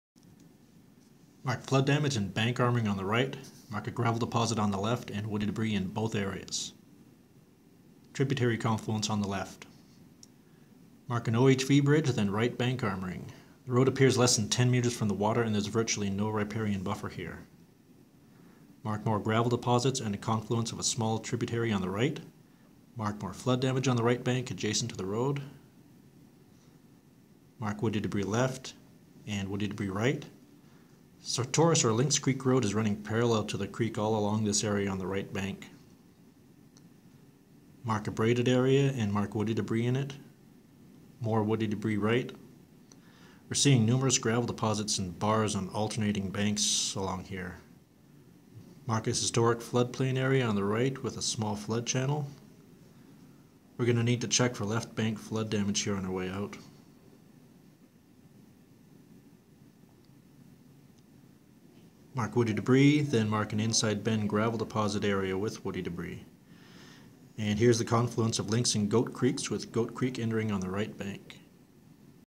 FISHES Program: Lynx Creek Project Aerial Footage
Preliminary habitat assessment for Alberta's Lynx Creek area.

Preliminary habitat assessments are conducted by staff using high definition aerial videography. Potential threats to fish and fish habitat are identified during the flight, then ranked and scored using a sophisticated computer model.

This project is part of the Southern Alberta Fisheries Habitat Enhancement and Sustainability (FISHES) Program.